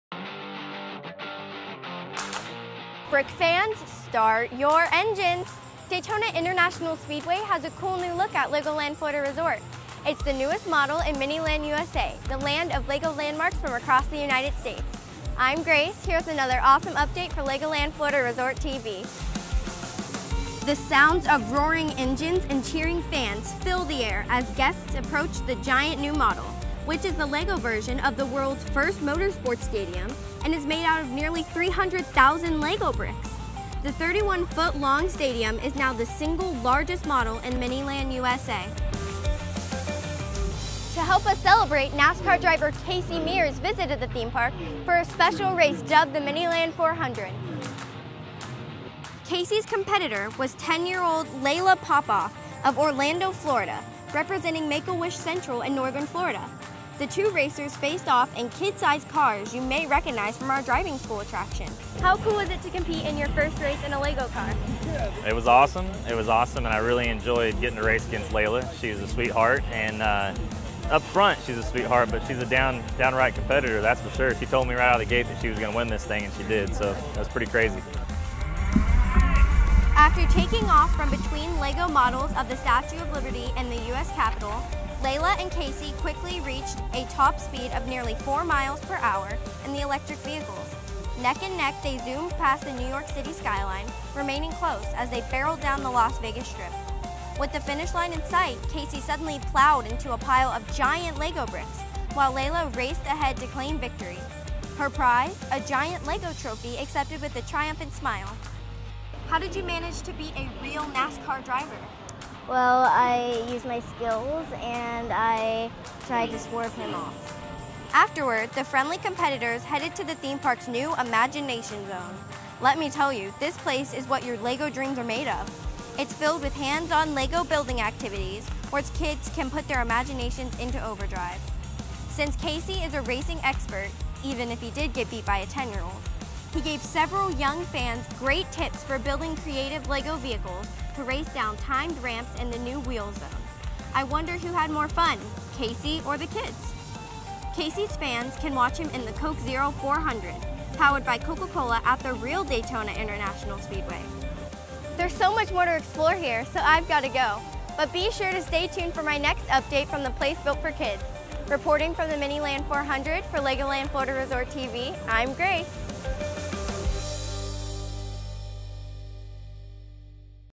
Make-A-Wish Winner Dominates MINILAND Race Against NASCAR's Casey Mears
On June 20, 2016, 10-year-old Layla Popoff of Orlando, Fla., zoomed to victory in the MINILAND 400 against veteran NASCAR driver Casey Mears, during a special event BuiltForKids to celebrate the giant new Daytona International Speedway model at LEGOLAND Florida Resort.

Layla was selected for the one-of-a-kind race with the assistance of Merlin’s Magic Wand, a global charity that has helped nearly 300,000 children and their families enjoy a magical day out at more than 100 attractions operated around the world by Merlin Entertainments, parent company of LEGOLAND.

Learn more about the LEGOLAND Florida Resort